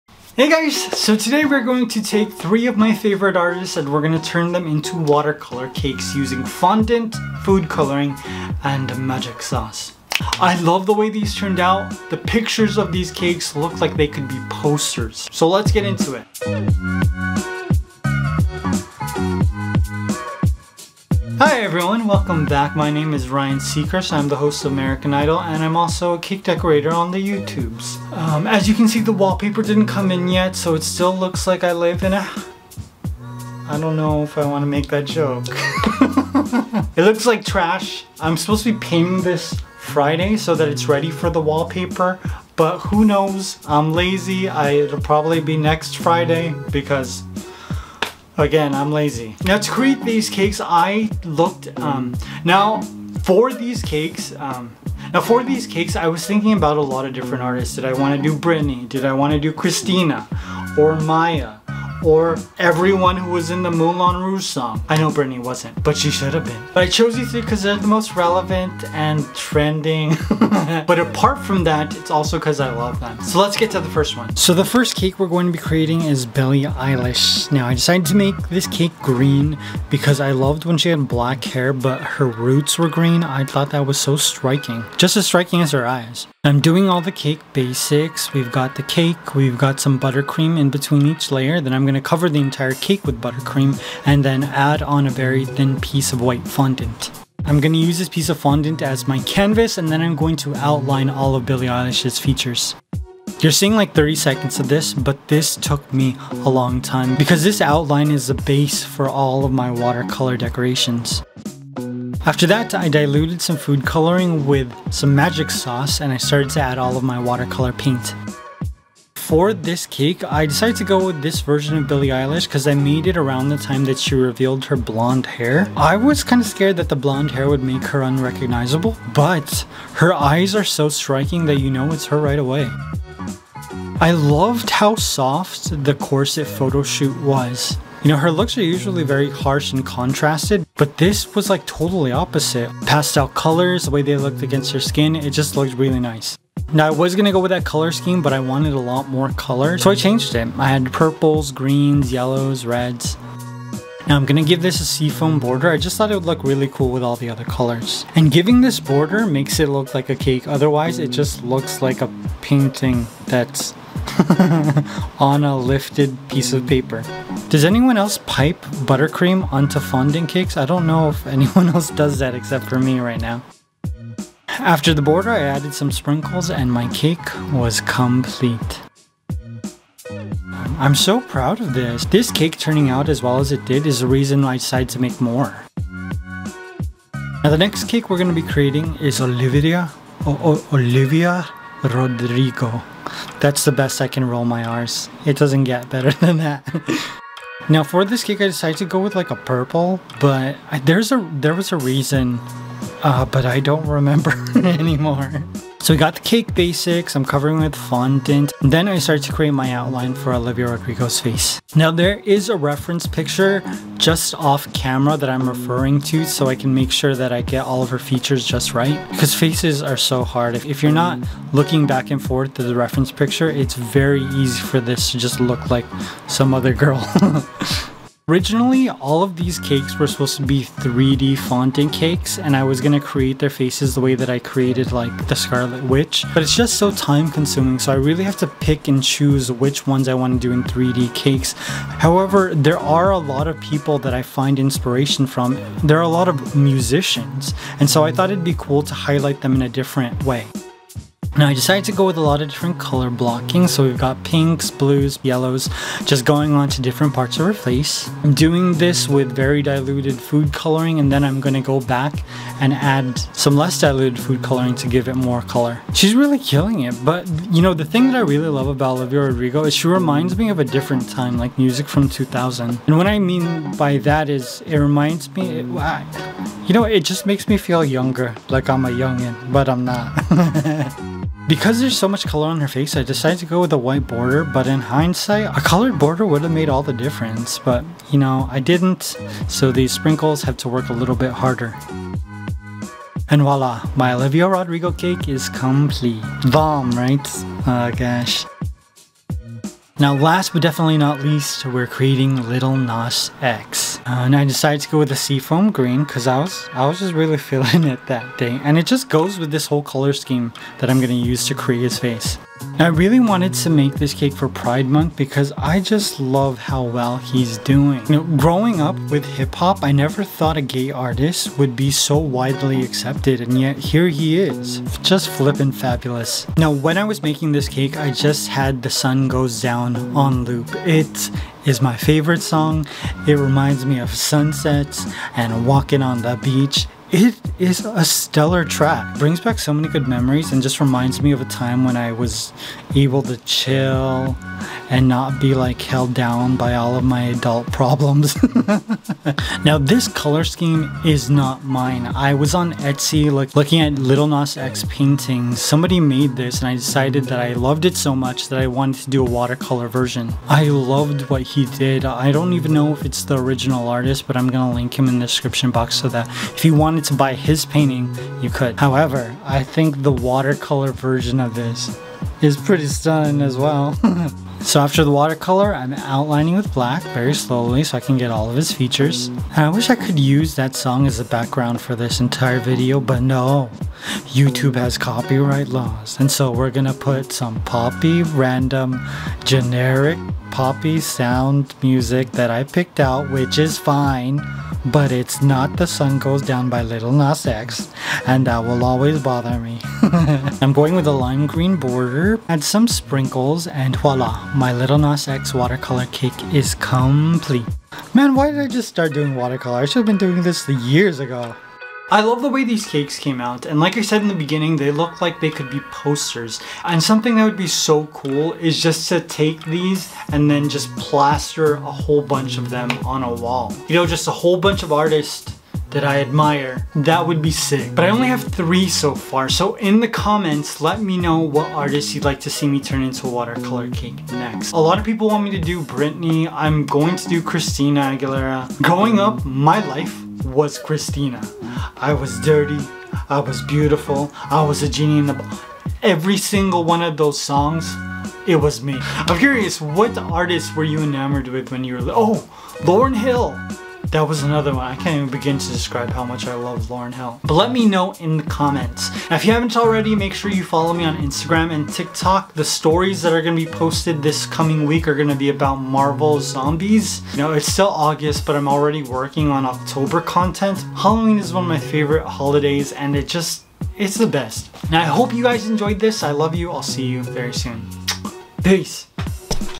3 Amazing Watercolor Cakes - Billie Eilish, Olivia Rodrigo, Lil Nas X
I created 3 Watercolor cakes to celebrate some of my favorite artists. Who should I make next? tSubscribe! goo.gl/dmvsyj

Can I decorate a                     Cake in under 30 min?